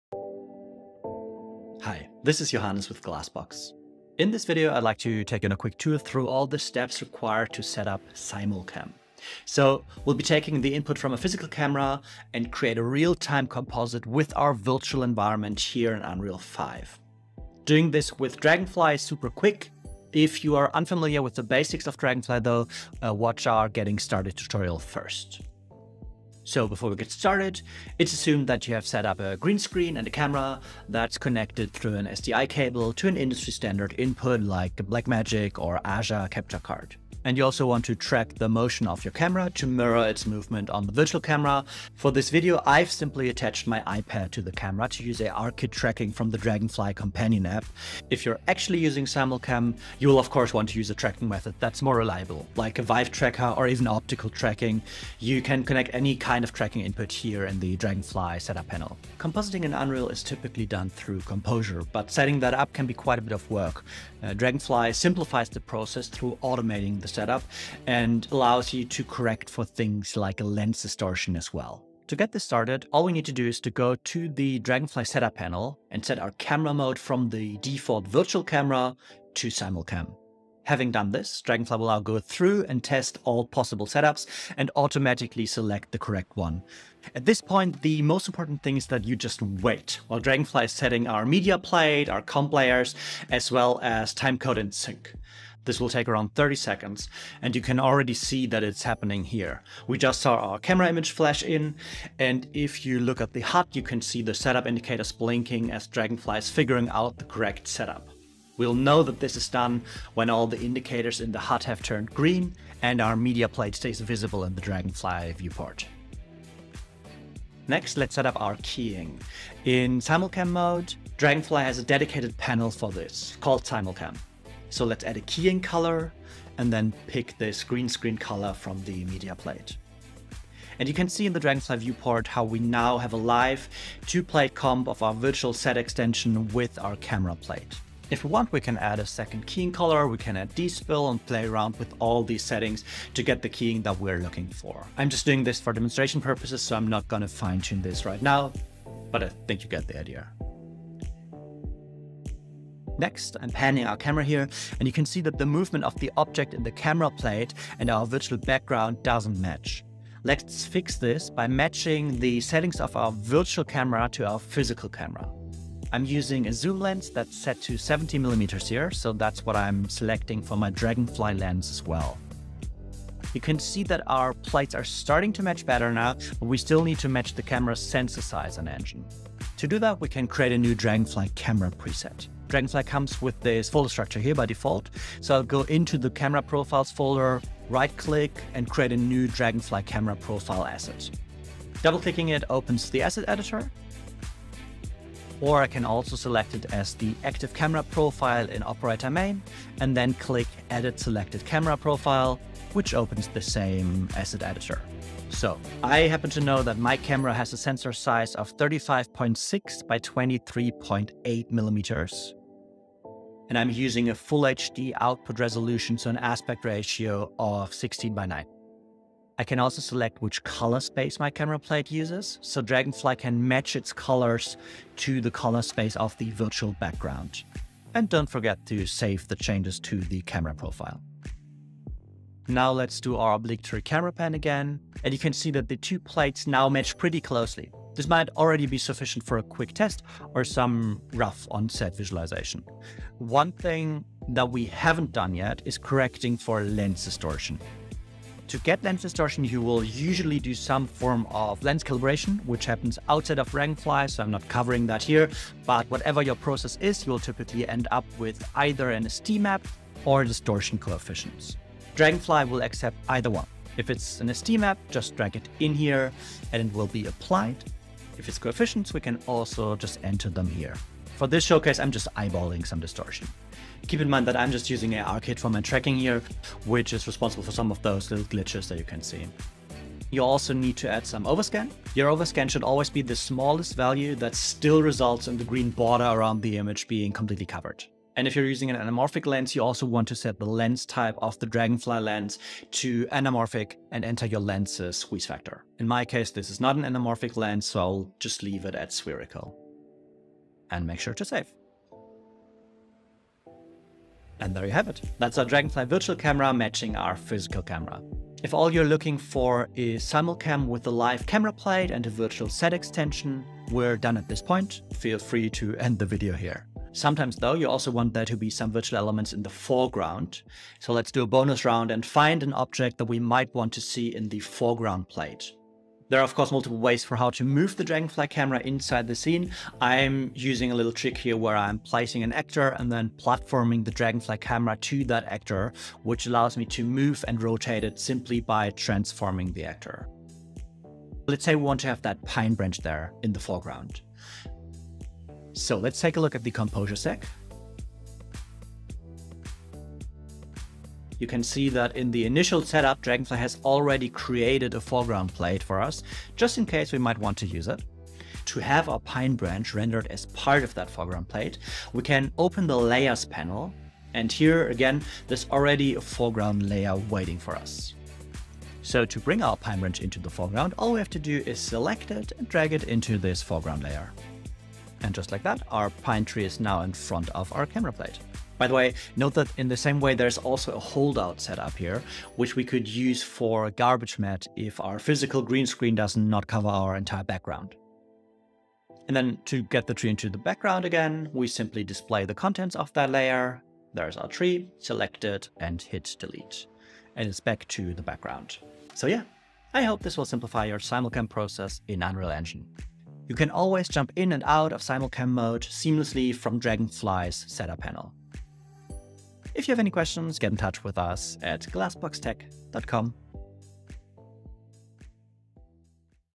DragonFly for Unreal: Simulcam Setup
A step-by-step walkthrough of how to set up Composure/Simulcam in unrealengine5 using our virtual camera, DragonFly by Glassbox Tech's Product Manager Johannes Wilke.

00:00 Introduction
00:26 Hardware Setup
00:59 Automatic Composure Setup
01:57 Chroma Key
02:32 Lens Settings
02:53 Sensor Settings
04:03 Lens Distortion
04:47 Anamorphic Lens Setup
05:16 Three-Layer Comp
06:18 Garbage Matte
06:38 Conclusion